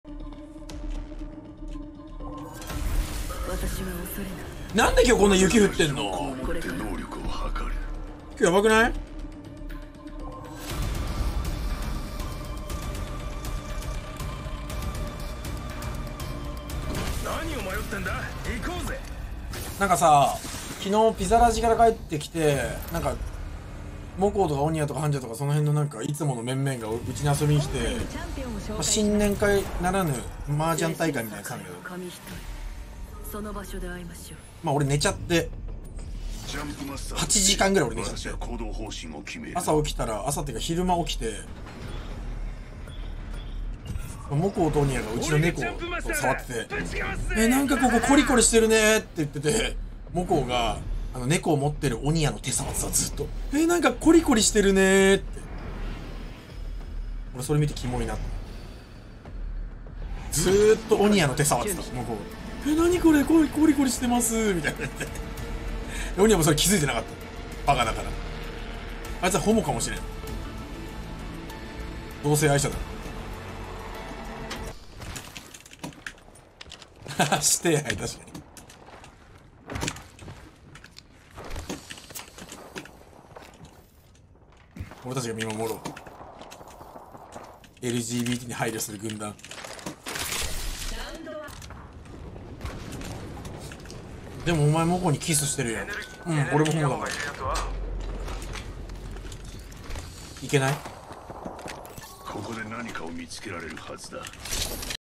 加藤純一宅でもこうとおにやのホモ現場を目撃してしまった件について
最新の配信の面白い部分、また過去配信の面白い部分や今でも残る伝説的シーンなど随時更新していきます。